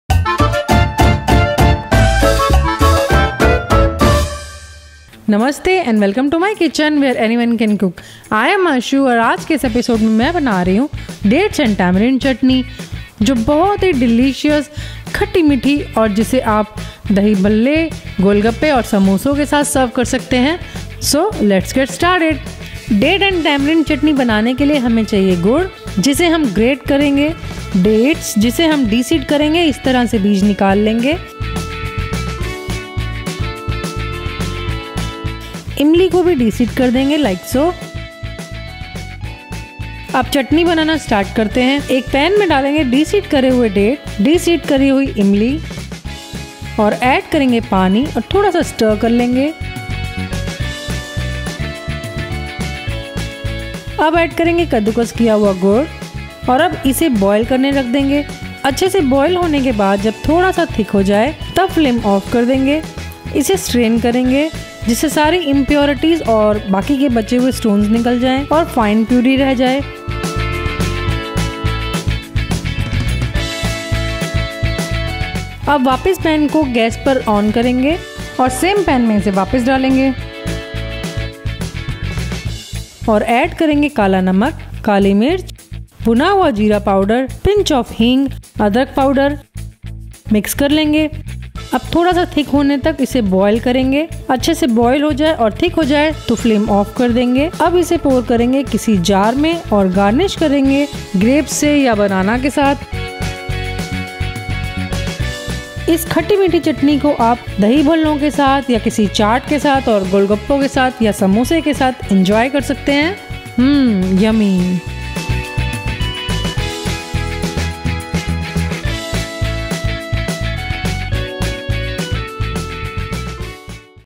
Tamarind Dates Chutney | खजूर इमली की चटनी | Khajur Imli ki Chutney
Tamarind Dates Chutney or Imli Khajur Ki chutney that is used for chaat and pani puri is a finger licking good chutney made with tangy tamarind, dates, sweet jaggery, and some Indian spices. It goes well with Dahi Bhalle(which will be uploaded tomorrow) or even samosas.
Learn how to make tamarind chutney for chaat or Imli ki Chutney in hindi now!

For more,

anyonecancookwithme.blogspot.in

Ingredients:
1/2 cup tightly packed tamarind (seedless)
1/2 cup seedless dates
1/2 cup jaggery grated
2 cup water
1/2 tsp ginger powder
1/2 tsp red chilli powder
1/2 tsp black salt
1/2 tsp jeera powder